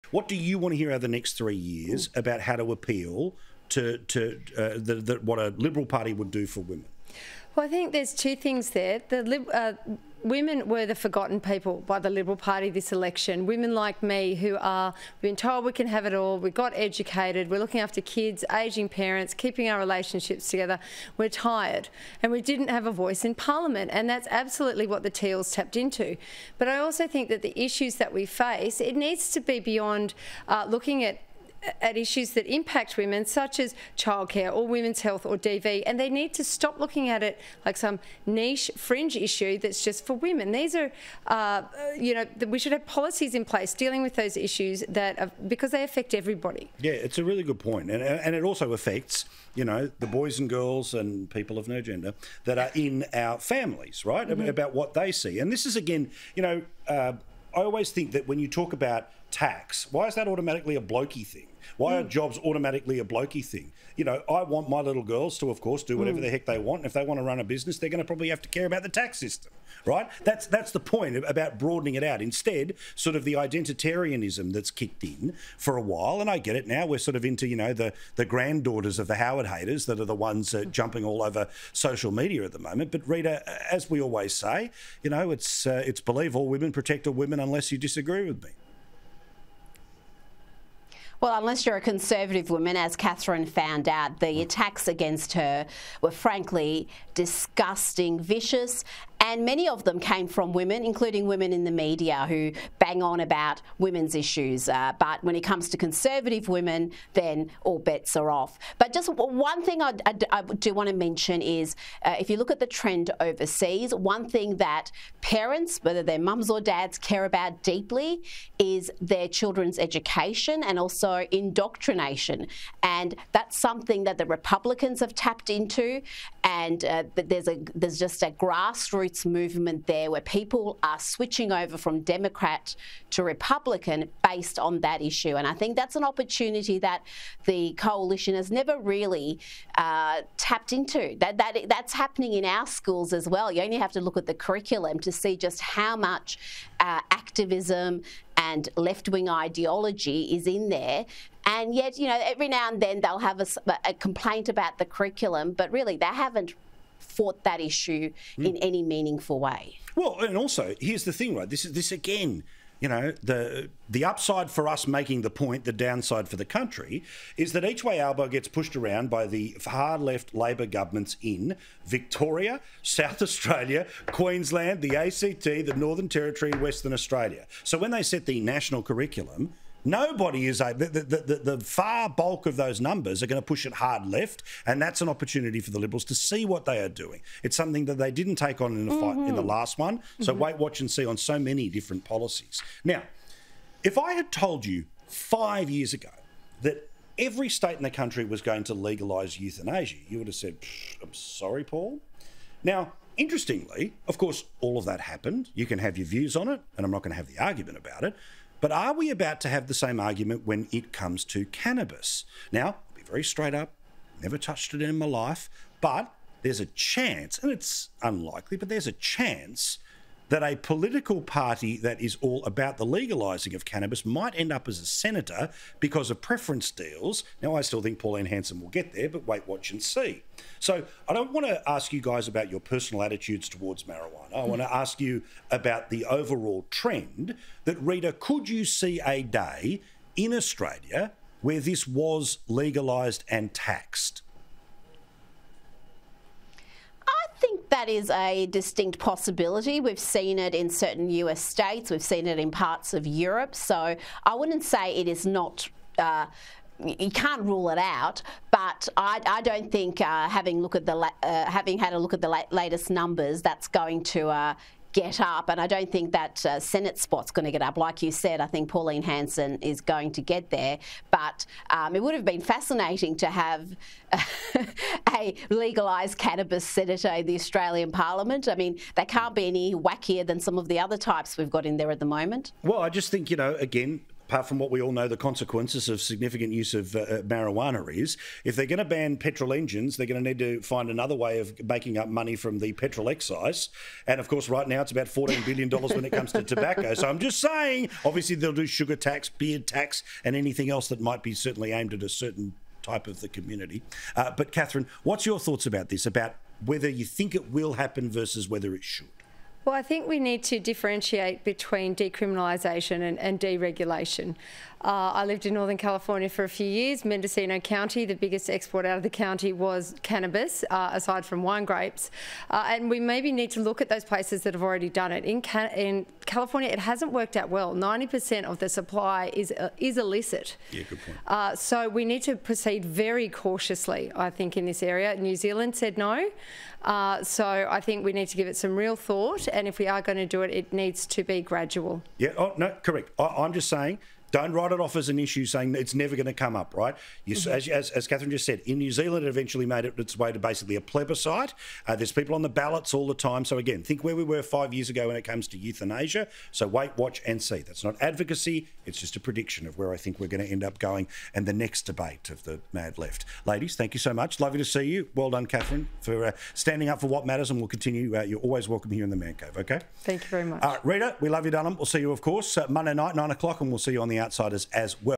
Women were ‘forgotten people’ by the Liberals this election
Former Liberal candidate for Warringah Katherine Deves says women were “the forgotten people” by the Liberal Party this election.

“Women like me who are told we can have it all – we got educated, we’re looking after kids, ageing parents, keeping our relationships together. We’re tired,” she told Sky News host Paul Murray.

“And we didn’t have a voice in parliament and that’s absolutely what the teals tapped into it.”